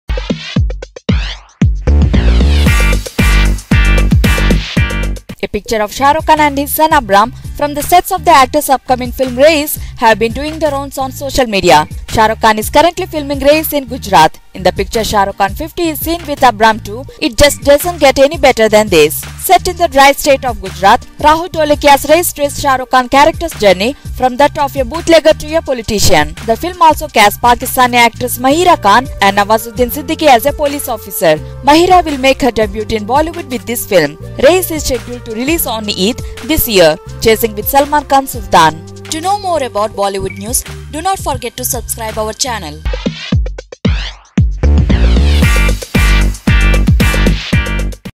Shah Rukh And AbRam on Raees Sets in Gujarat
A picture of Shah Rukh Khan and his son AbRam from the sets of the actor's upcoming film Raees have been doing the rounds of social media. Shah Rukh is currently filming Raees in Gujarat.Set in the dry state of Gujarat, Rahul Dholakia's Raees traces SRK's character's journey from that of a bootlegger to a politician. The film also casts Pakistani actress Mahira Khan and Nawazuddin Siddiqui as a police officer. Mahira will make her debut in Bollywood with this film.